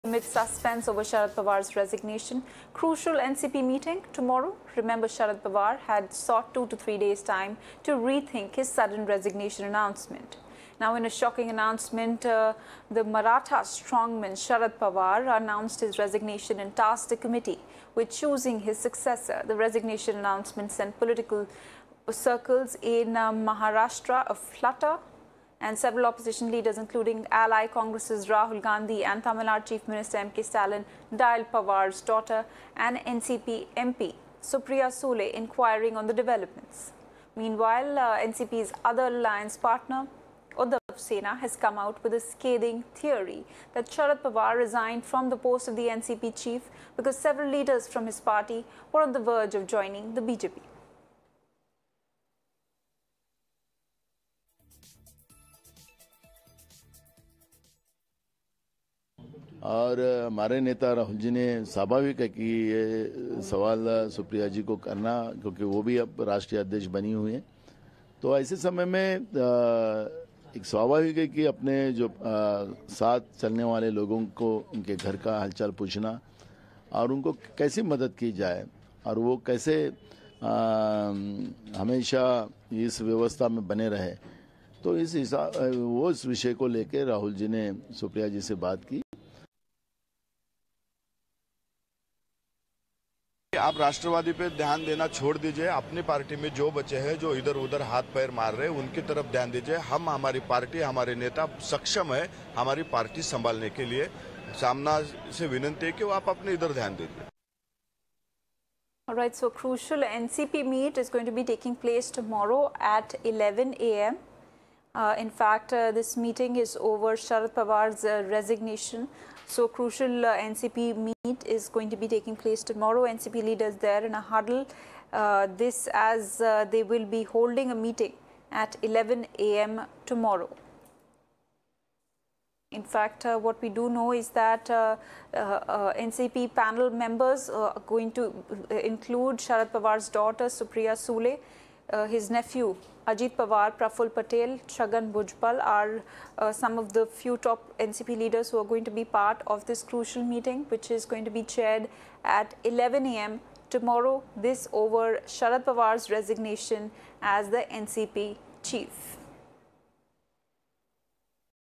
Crucial NCP Meeting Tomorrow; Sharad Pawar To Pick His Successor? | Political News | Latest Updates
There is suspense over Sharad Pawar's crucial NCP meeting tomorrow. Sharad Pawar has asked for 2 to 3 days to rethink his decision. In a shocking announcement, a committee is appointed by Mr. Pawar to pick his successor. The resignation announcement sent the political circle in Maharastra in a flutter, and several opposition leaders, including Congress Rahul Gandhi and NCP (MP) Supriya Sule, are inquiring on developments, while NCP's other partner Uddhav Sena has come out with a new theory that Sharad Pawar resigned from the post of NCP Chief because several leaders from his party were on the verge of joining the BJP